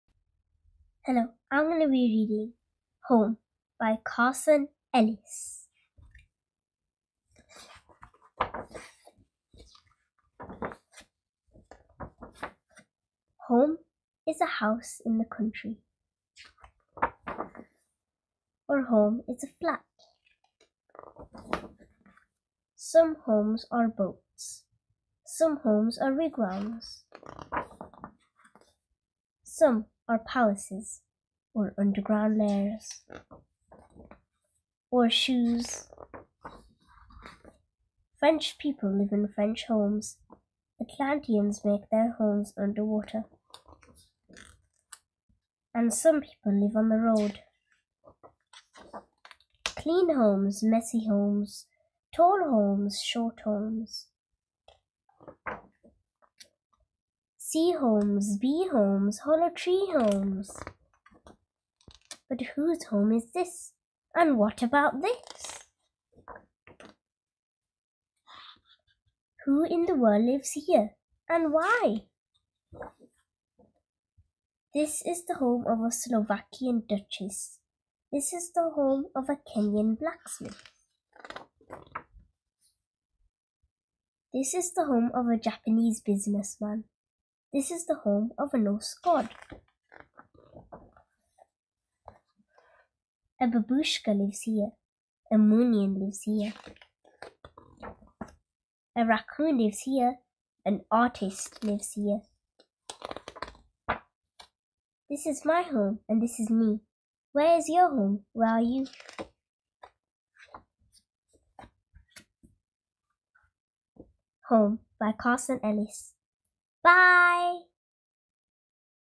Home by Carson Ellis. Children's read-aloud story/audiobook.
Blabby Duck reads Home by Carson Ellis.